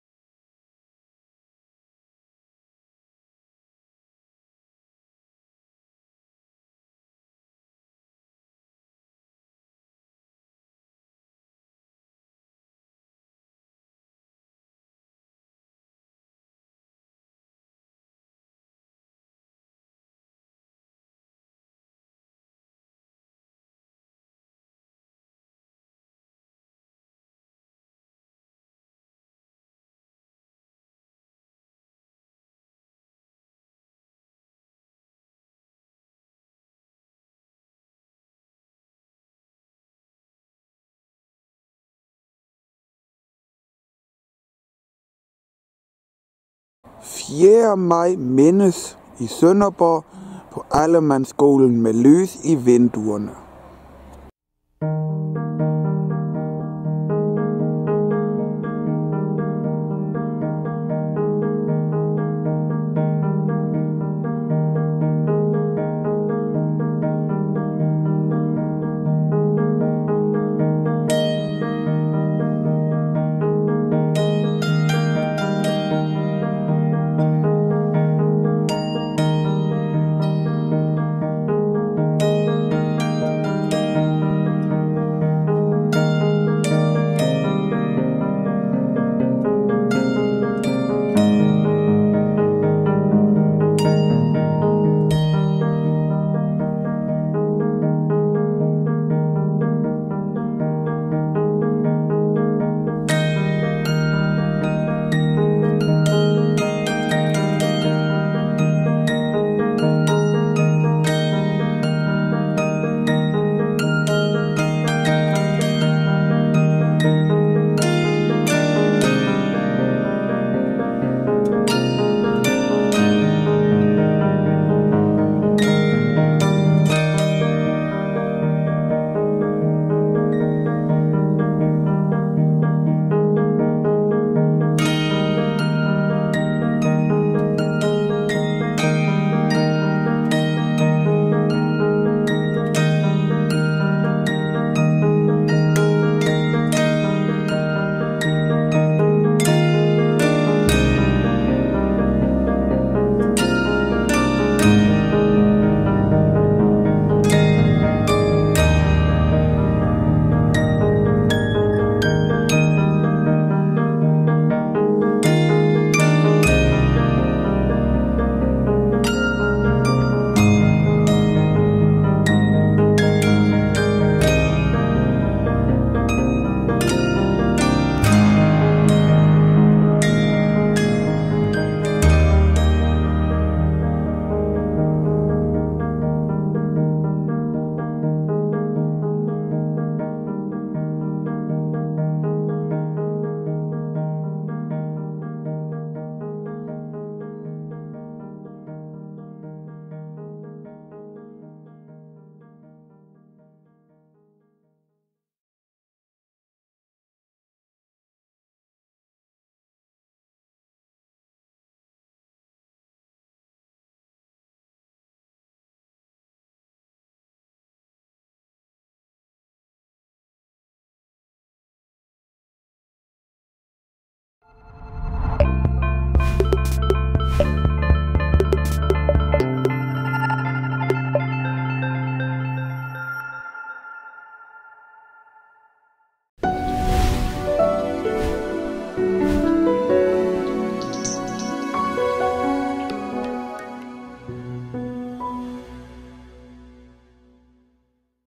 4. med lys i vinduer på ahlmann-skolen (HD 1080p50)
På Ahlmann-Skolen blev befrielsen også markeret med levende lys i vinduerne ud mod Kongevej og det er en frivillig flok af ansatte, der var gået sammen om at holde traditionen i live.